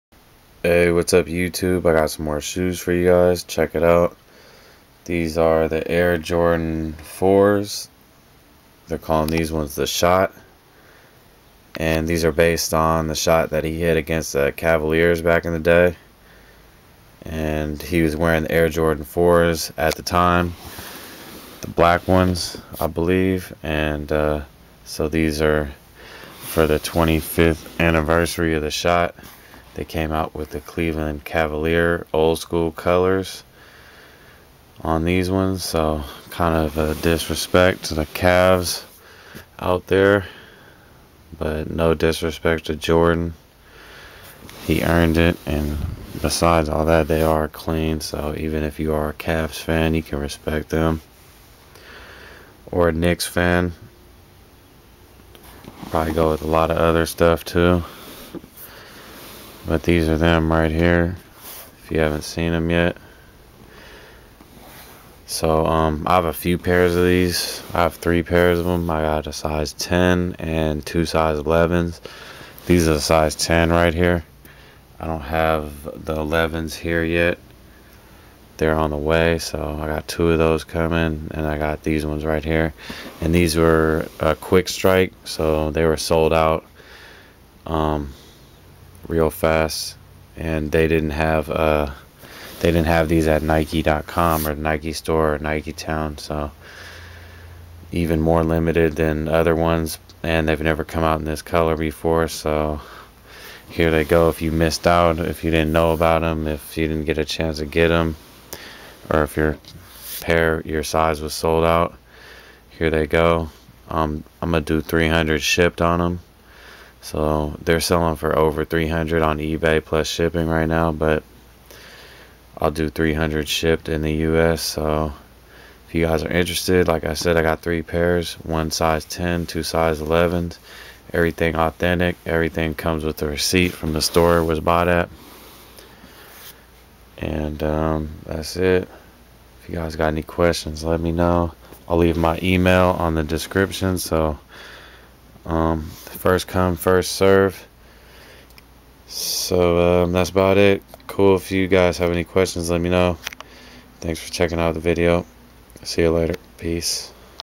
Nike Air Jordan IV 4 Retro "The Shot" Cavs / Knicks colors. size 11 $270 Shipped
Nike Air Jordan IV 4 Retro "The Shot" Cavs / Knicks colors. size 10 & 11 $270 Shipped in the US. I have 2 size 11. 100% authentic with receipt.